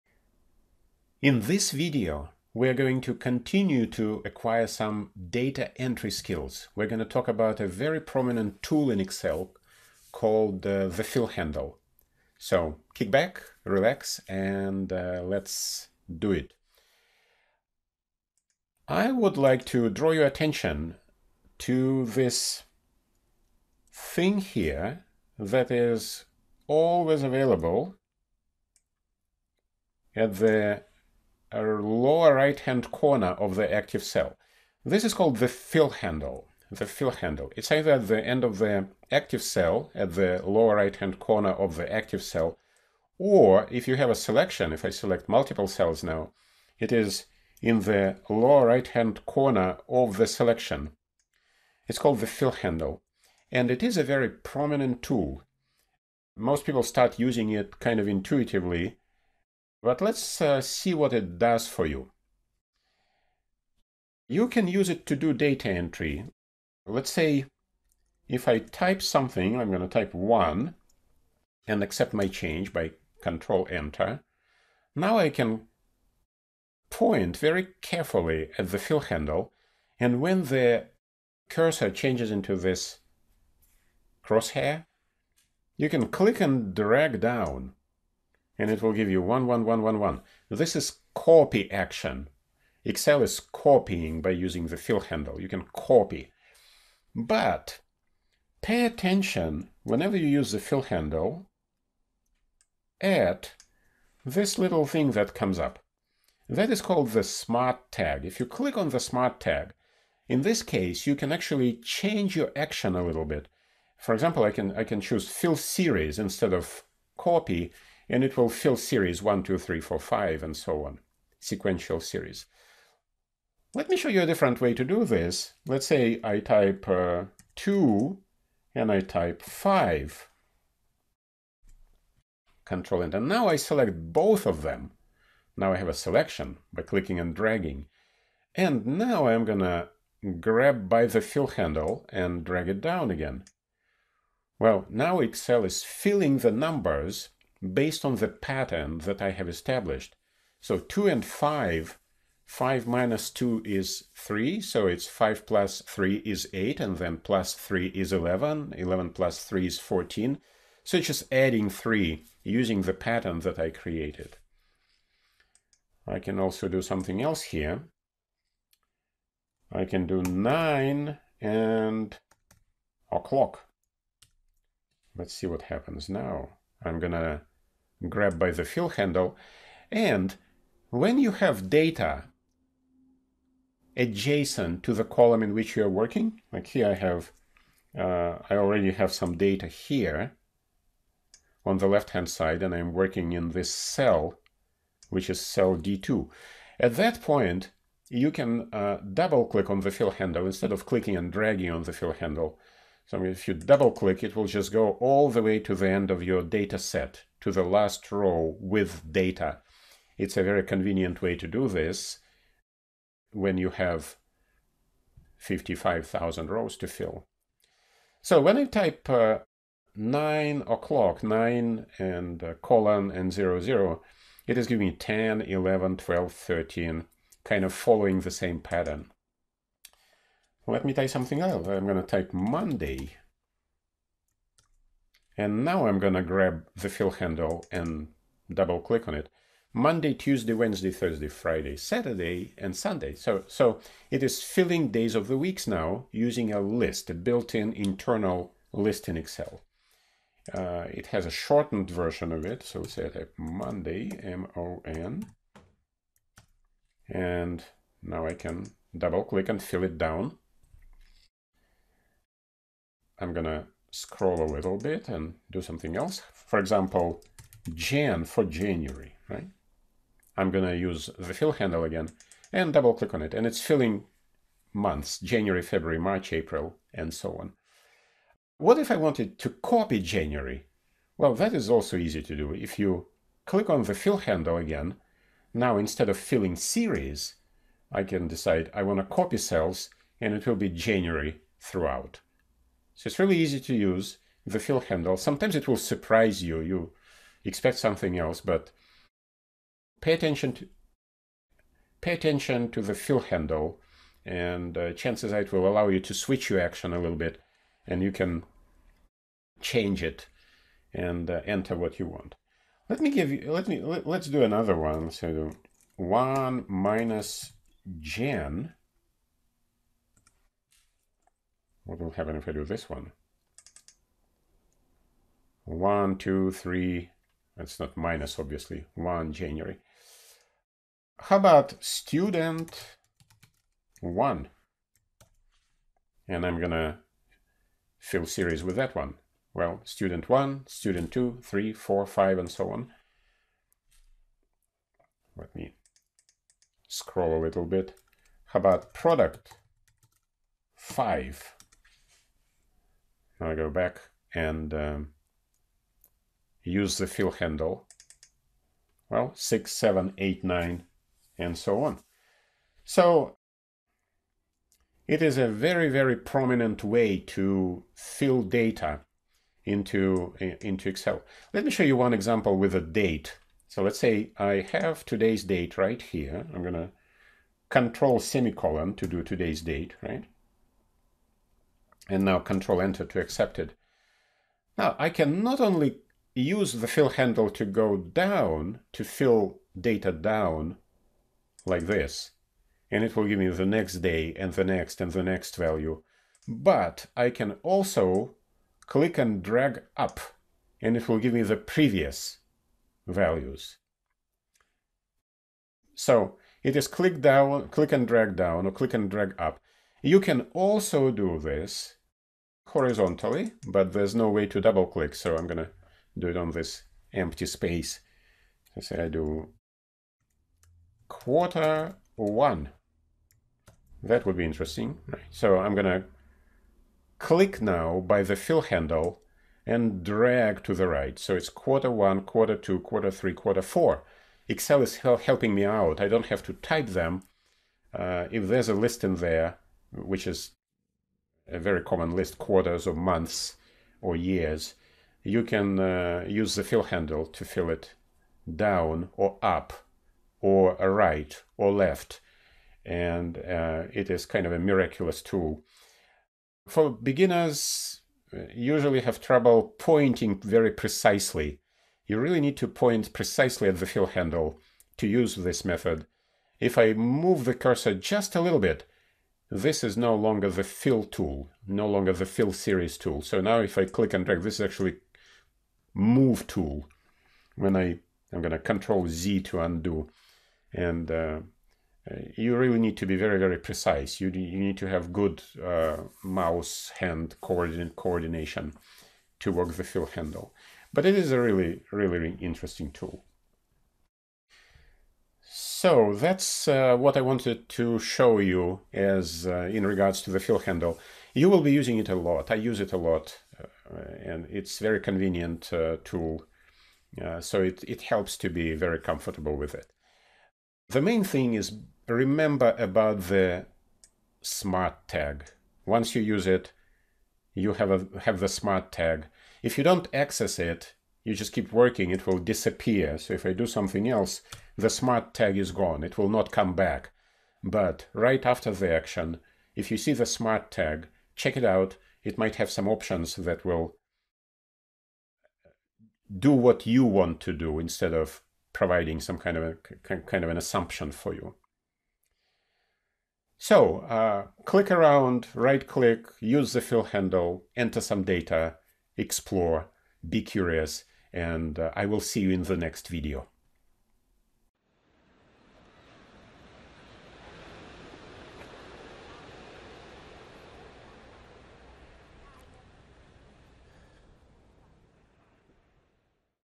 Excel 2013 Tutorial: Data Entry With Fill Series in Microsoft Excel
Here you will learn some tips and tricks using the Fill-Handle or fill series in Excel.  Enter data in Excel - the right way!

Want to learn Excel?